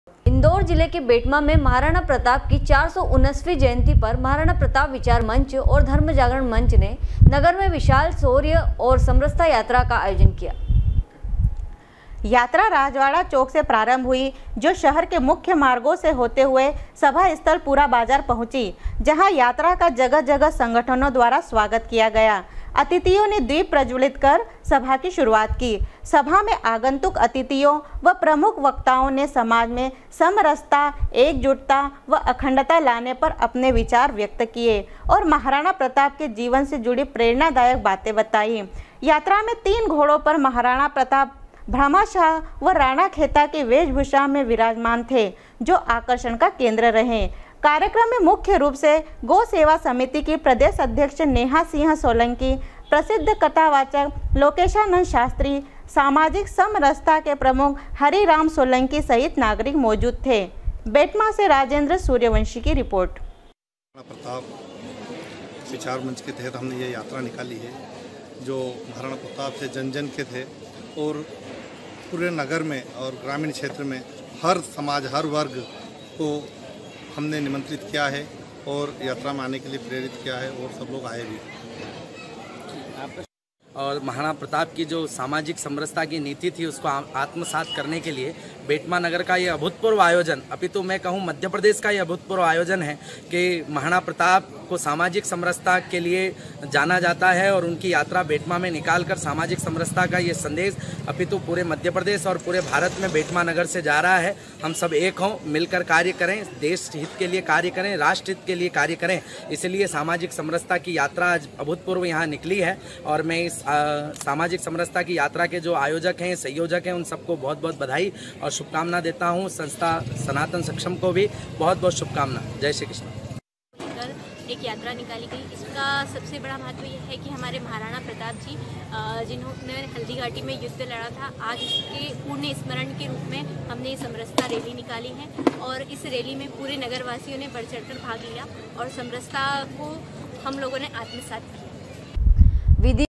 sw24news betma, 31.05.2019
इन्दौर जिले के बेटमा मंे महाराणा प्रताप की 479 वीं जयंती पर महाराणा प्रताप विचार मंच ओर धर्म जागरण मंच ने नगर मे विशाल शौर्य ओर समरसता यात्रा का आयोजन किया